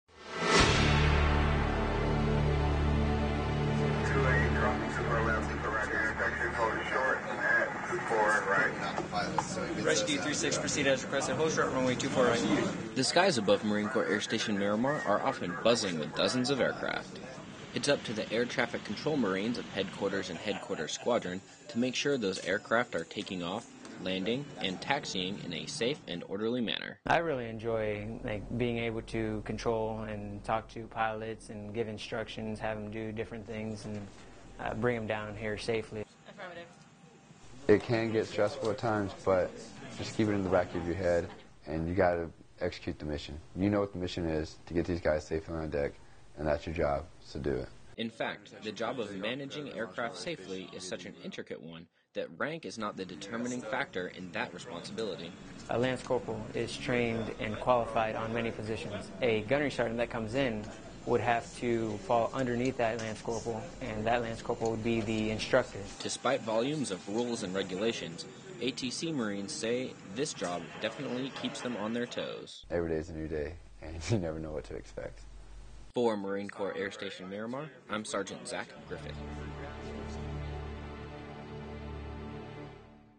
Air traffic control Marines keep airfield, pilots safe
Air traffic control Marines use their training to safely guide pilots throughout the busy airways and landing strips of Marine Corps Air Station Miramar, California. Sgt. Zach Griffith brings us the story.